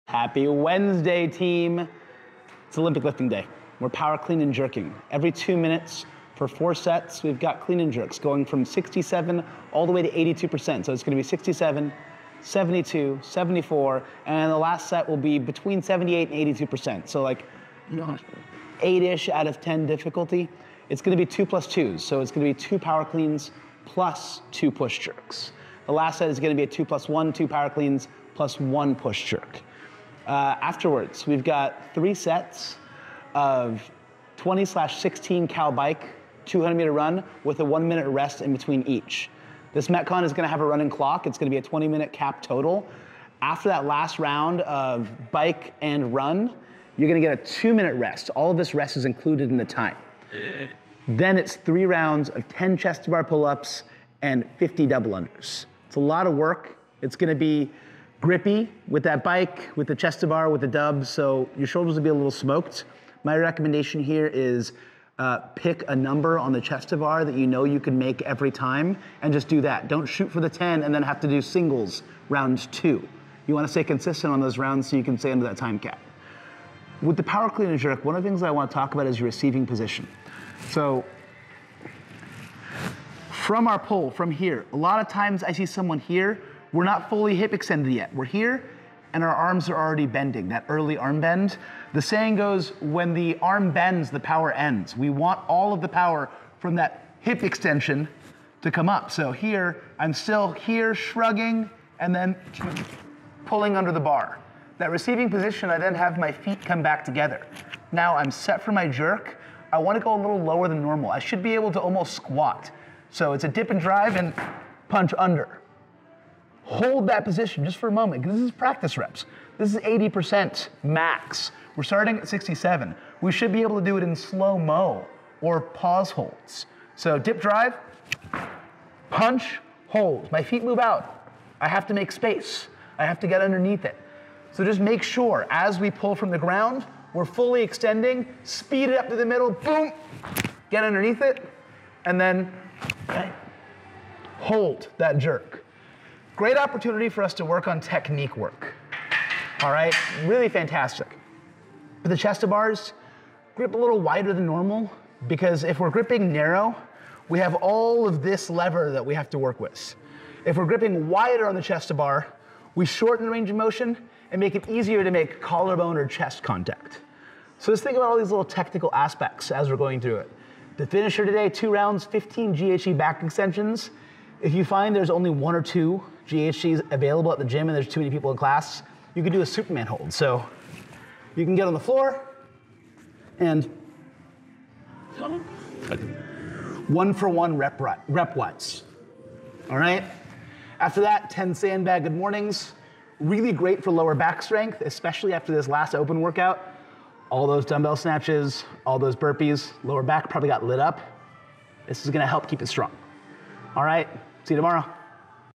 Workout of the Day 03/06/2024
Warmup
:90 Jump Rope Practice
5 Inch Worms
10 Wall Extensions
:30 Supinated Hang from bar
10/10 Greatest Stretch Rotations
10 Squat Jumps
10 Good Mornings
5 Behind the Neck Press
5 Deadlift to Shrug
5 Muscle Cleans + 5 Press
5 Powe Cleans
5 Push Jerks

Strength
Power Clean and Jerk
Every 2:00 x 8 Sets
2x(2+2) @67%
2x(2+2) @72%
2x(2+1) @74%
2x(2+1)@78-82%

*These are not touch and go reps.Take a full reset between cleans.
**Use RPE as a guide, newer athletes can build

Metcon
3 Sets:
20/16 Cal Bike
200m Run
Rest 1:00 between sets

Rest 2:00 after the last set and then.

3 Rounds
10 C2B
50 Double-Unders

20min cap

L1: 12/8 Cals, 100m run, Ring Rows, Single Unders
L2: 15/12 Cals, Drag Rope singles or 25 DUs or :60 struggle Unders
Rx+: 5 Rds on part 2 w/ 35 Crossover Single Unders

Finisher
2 Sets
15 GHD Back Extensions
10 Sandbag Good Mornings
*rest 2:00 between sets